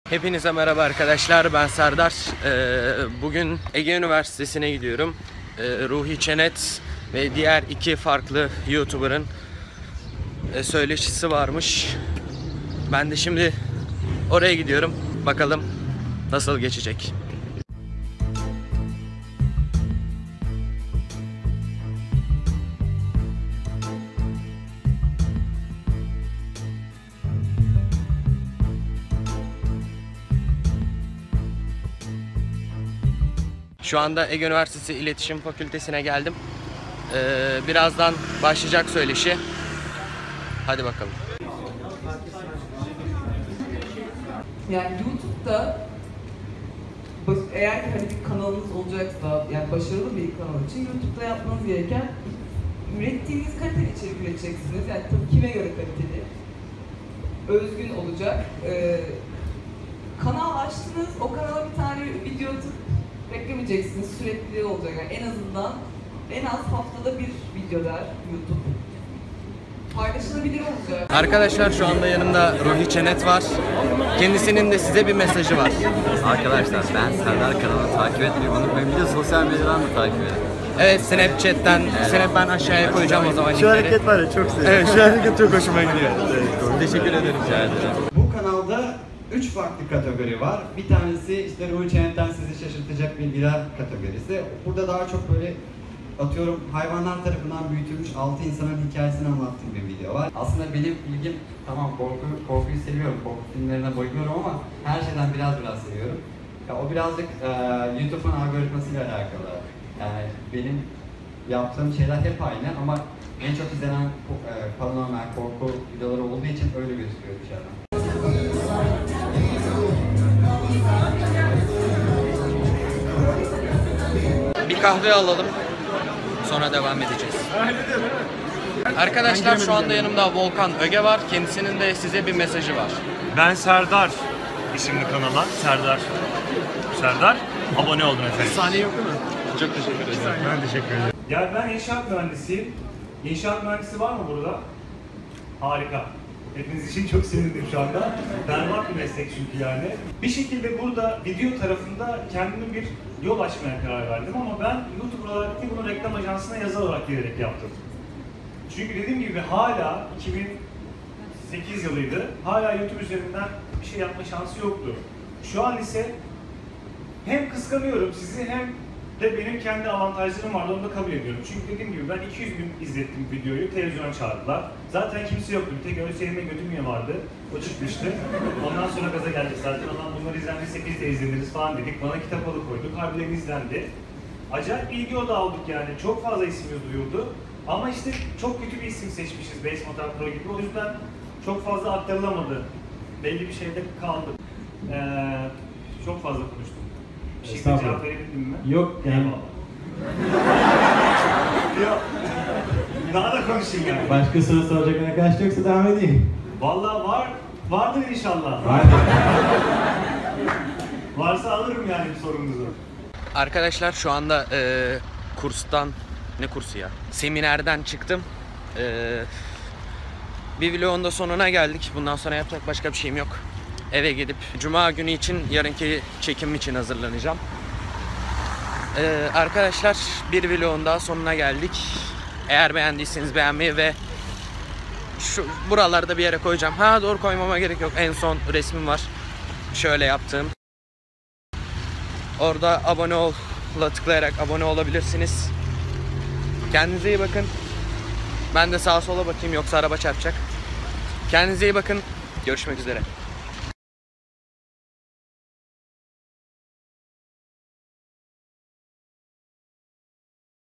VLOG #2 - Ruhi Çenet ve Volkan Öge'nin Sizlere Mesajı Var
Ruhi Çenet ve Volkan Öge'nin güzel mesajları ve verdikleri güzel bilgileri için kendilerine çok teşekkür ediyorum :)